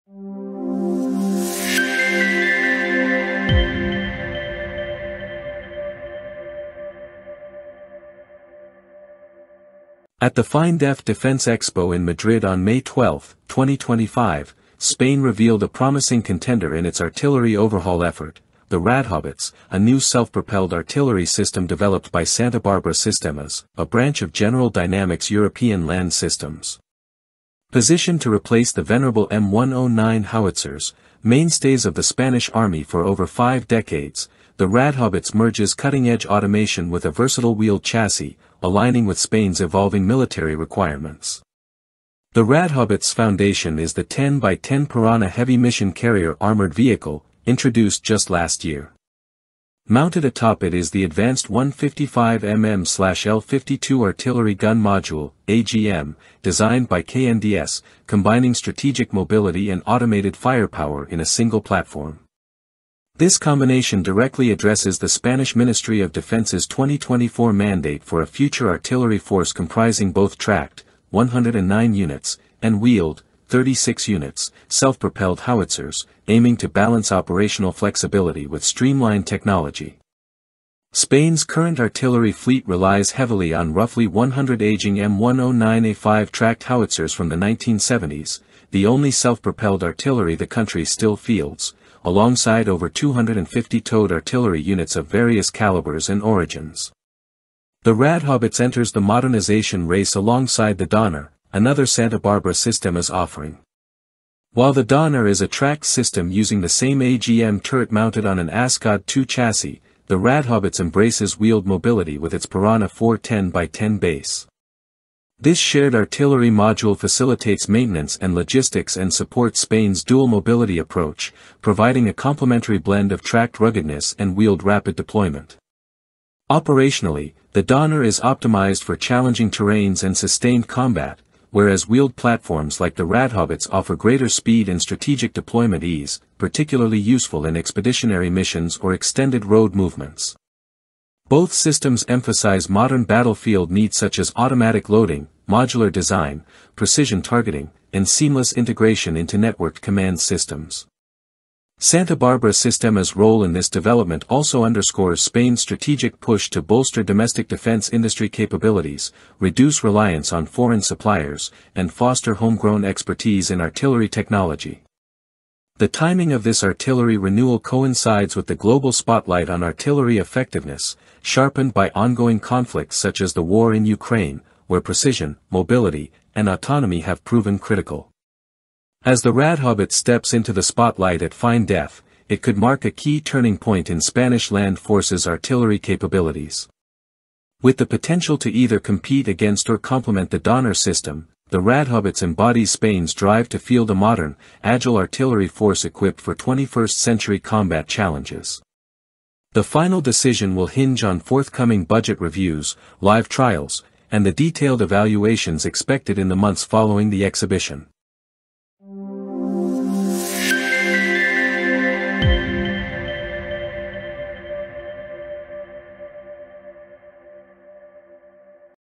Spain unveiled the Radhaubitze, a top contender to replace 100 obsolete M109s in the army
The article details Spain’s unveiling of the Radhaubitze self-propelled artillery system at Feindef 2025, highlighting its role as the leading candidate to replace the aging M109 howitzers. It covers technical specs, operational advantages, and Spain’s artillery modernization plans.